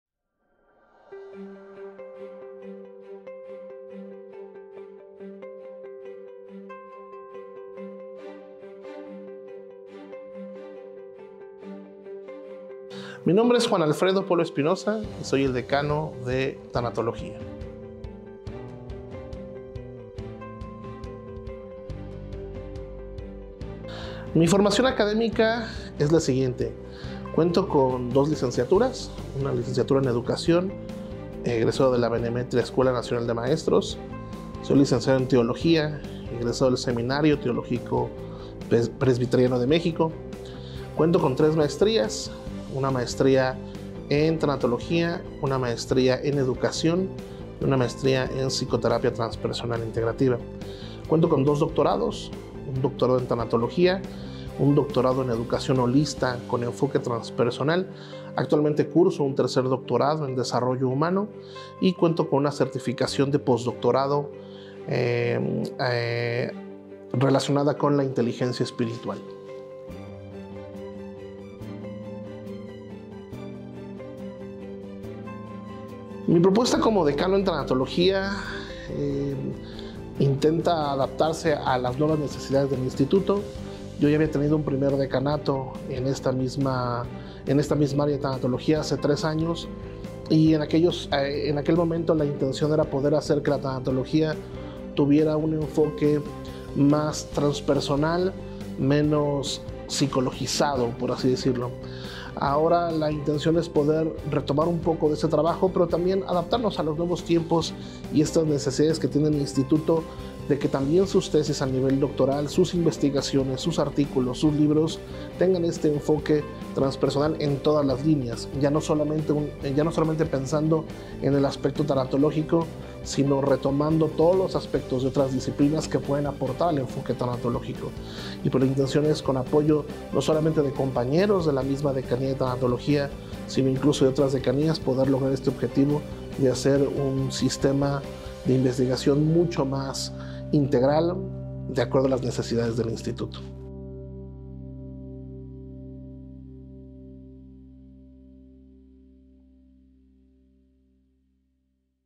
Conoce a tu decano el Dr. Juan Alfredo Polo Espinosa
Comunidad IMPo:
Te invitamos a conocer a tu Decano el Dr. Juan Alfredo Polo Espinosa, Decano de Tanatología.